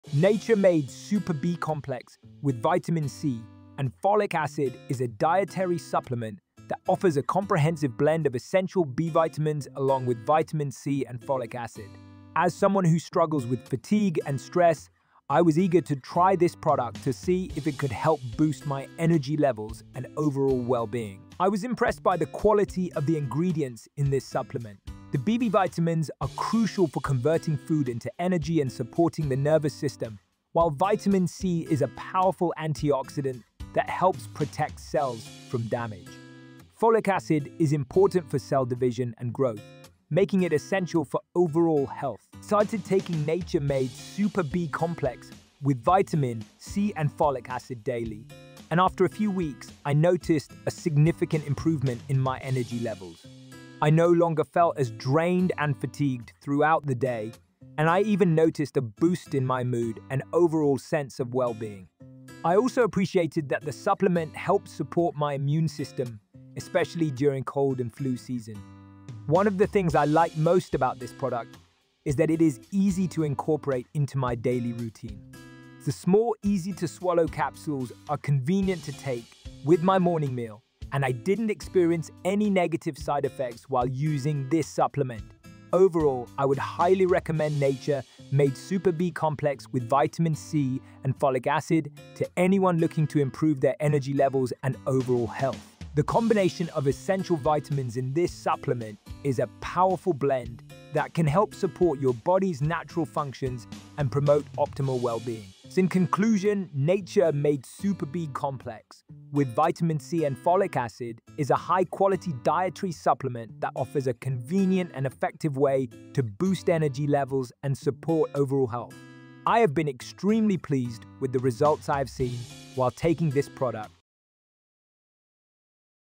Nature Made Super B Complex with Vitamin C and Folic Acid Dietary Review
Nature Made Super B Complex with Vitamin C and Folic Acid is a dietary supplement that offers a comprehensive blend of essential B vitamins along with vitamin C and folic acid. As someone who struggles with fatigue and stress, I was eager to try this product to see if it could help boost my energy levels and overall well-being.

I was impressed by the quality of the ingredients in this supplement. The B vitamins are crucial for converting food into energy and supporting the nervous system, while vitamin C is a powerful antioxidant that helps protect cells from damage. Folic acid is important for cell division and growth, making it essential for overall health.

I started taking Nature Made Super B Complex with Vitamin C and Folic Acid daily, and after a few weeks, I noticed a significant improvement in my energy levels. I no longer felt as drained and fatigued throughout the day, and I even noticed a boost in my mood and overall sense of well-being. I also appreciated that the supplement helped support my immune system, especially during cold and flu season.

One of the things I liked most about this product is that it is easy to incorporate into my daily routine. The small, easy-to-swallow capsules are convenient to take with my morning meal, and I didn't experience any negative side effects while using this supplement.

Overall, I would highly recommend Nature Made Super B Complex with Vitamin C and Folic Acid to anyone looking to improve their energy levels and overall health. The combination of essential vitamins in this supplement is a powerful blend that can help support your body's natural functions and promote optimal well-being.

In conclusion, Nature Made Super B Complex with Vitamin C and Folic Acid is a high-quality dietary supplement that offers a convenient and effective way to boost energy levels and support overall health. I have been extremely pleased with the results I have seen while taking this product.